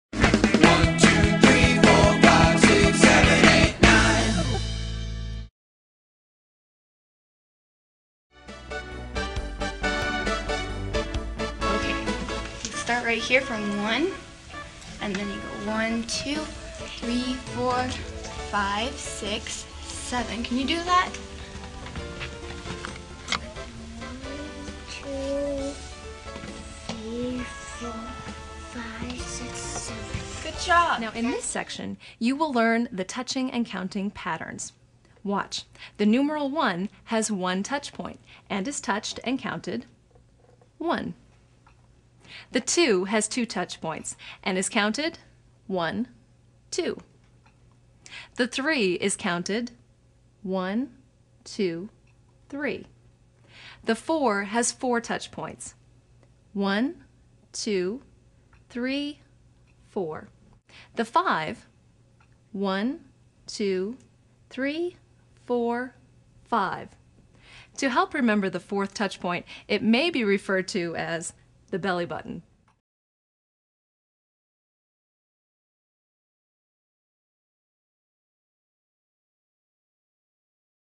Touching/Counting Patterns
TeacherTube User: Touchmath

The secret to TouchMath is simple. In fact it can be summed up in one word TouchPoints. Students count aloud as they touch the single TouchPoints once and double TouchPoints twice. This multisensory approach engages students on auditory visual and tactile/kinesthetic levels.